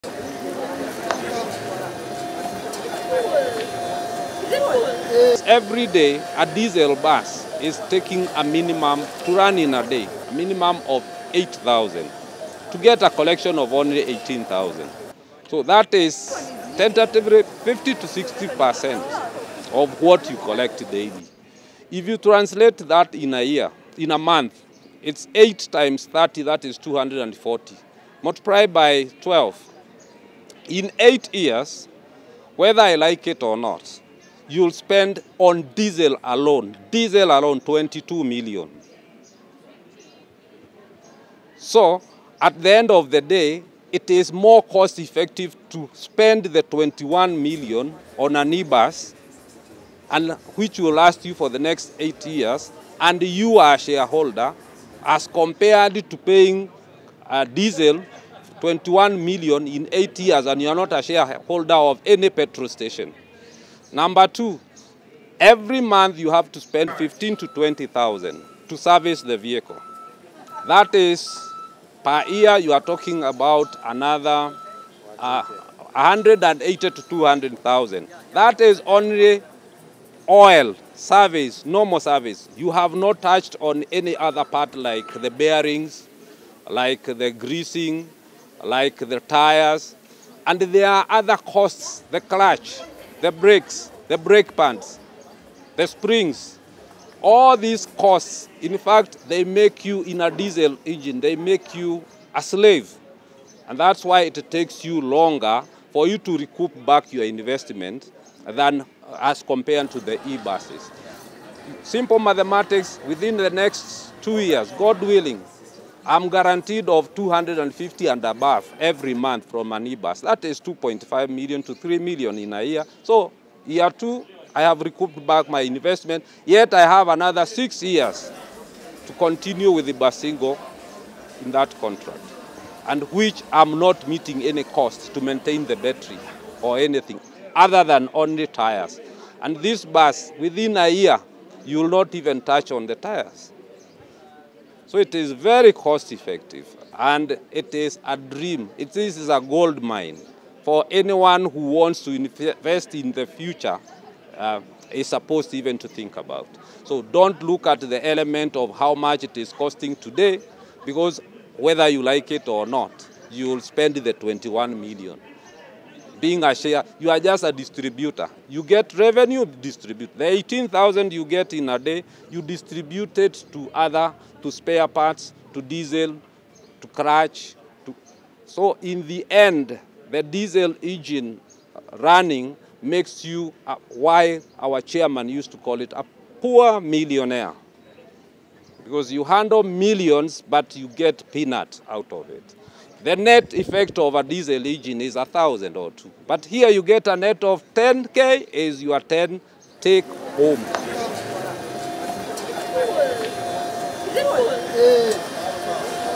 Electric Buses in Kenya: Going, Green
‘Diesel buses made us POOR MILLIONAIRES’ PSV’s Operators say as they embrace ELECTRIC BUSES.
Battery electric buses range from 1.4 to 7.7 times better than diesel bus, as shown in miles per gallon emissions-equivalency. Another way of saying this is that a diesel bus has nearly 1½ to 8 times the global warming emissions as an electric bus, depending on the region.